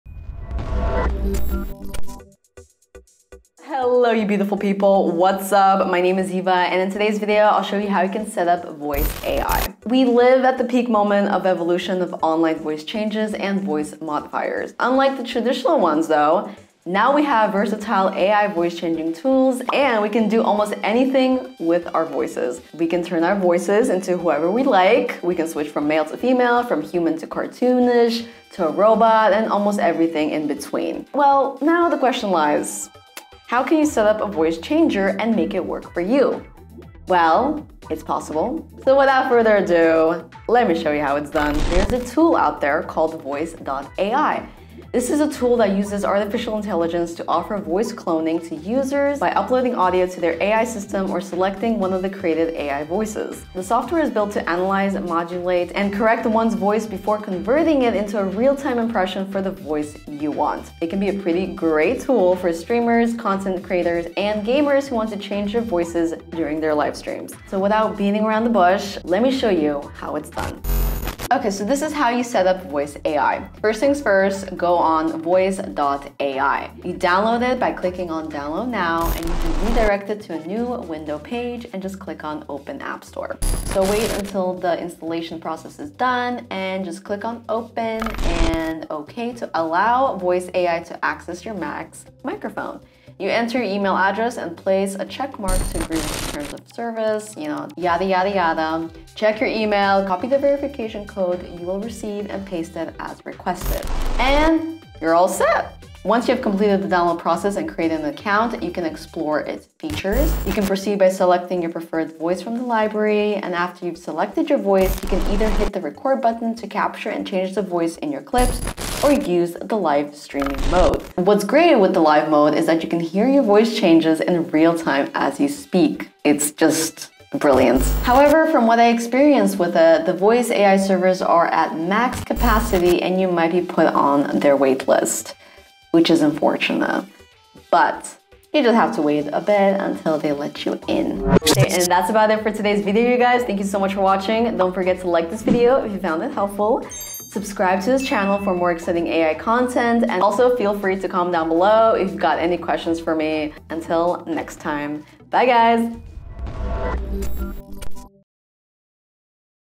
How to Set Up Voice.ai
Embark on a thrilling journey into the world of Voice AI with this comprehensive tutorial! From basic setup to advanced tricks, we've got you covered. Tap into this kind of world for a bit and don't forget, just have fun with it!